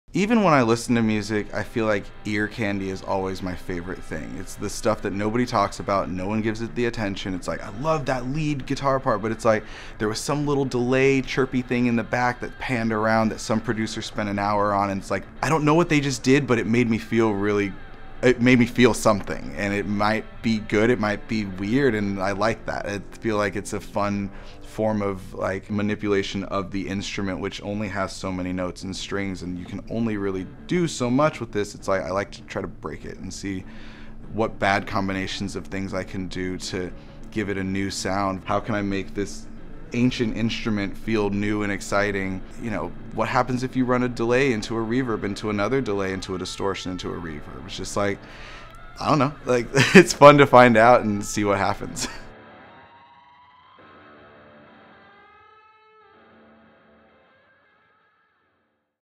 Discovering new sounds
Izzy Fontaine shares about how he pushes the envelope of guitar playing to create mesmerizing ear candy